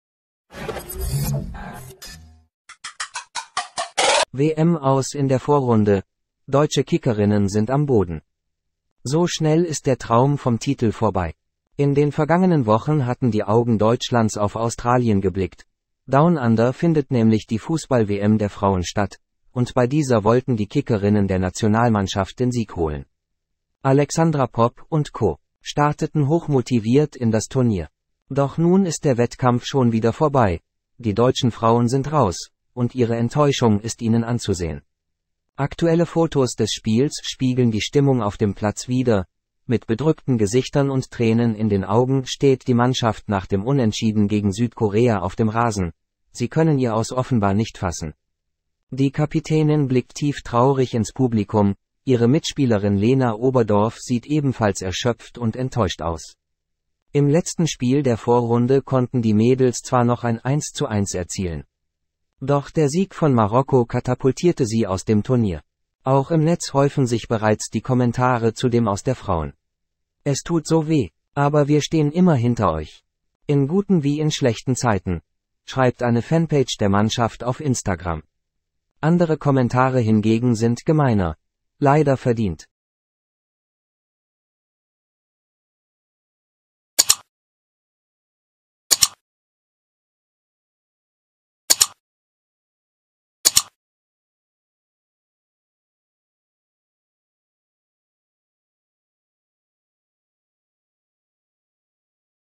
WM-Aus in der Vorrunde: Deutsche Kickerinnen sind am Boden germany | SH News German
So schnell ist der Traum vom Titel vorbei! In den vergangenen Wochen hatten die Augen Deutschlands auf Australien geblickt: Down Under findet nämlich die Fußball-WM der Frauen statt – und bei dieser wollten die Kickerinnen der Nationalmannschaft den Sieg holen. Alexandra Popp und Co. starteten hochmotiviert in das Turnier. Doch nun ist der Wettkampf schon wieder vorbei: Die deutschen Frauen sind raus – und ihre Enttäuschung ist ihnen anzusehen!